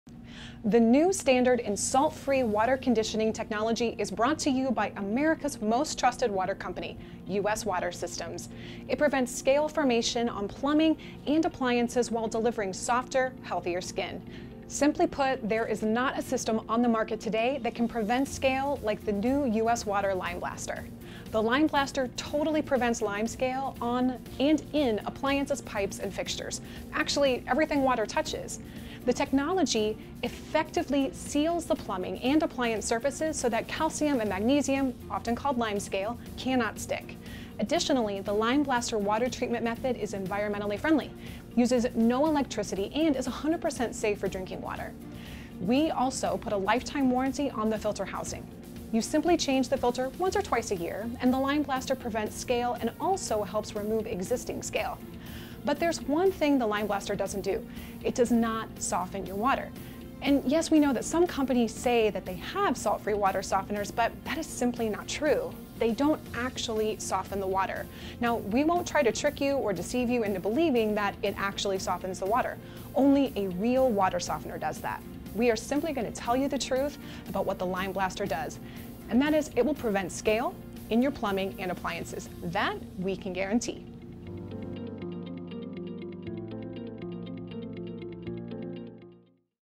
Remove Scale from your Water, Salt Free
It prevents scale formation on plumbing and appliances while delivering softer, healthier skin. There is not a system on the market today that can prevent scale like the new US Water LimeBlaster - pure and simple!

The US Water Limeblaster is the new revolutionary and economic way to deal with hard water. The US Water Limeblaster totally prevents limescale on and in appliances, pipes, fixtures - actually everything water touches. Our new Limeblaster System is the new standard by which all salt free water conditioners will ultimately be judged. The new Limeblaster technology effectively "Seals" the plumbing and appliance surfaces so that calcium and magnesium (often called limescale) cannot stick. Additionally, the Limeblaster water treatment method is environmentally friendly, uses no electricity and is 100% safe for drinking water.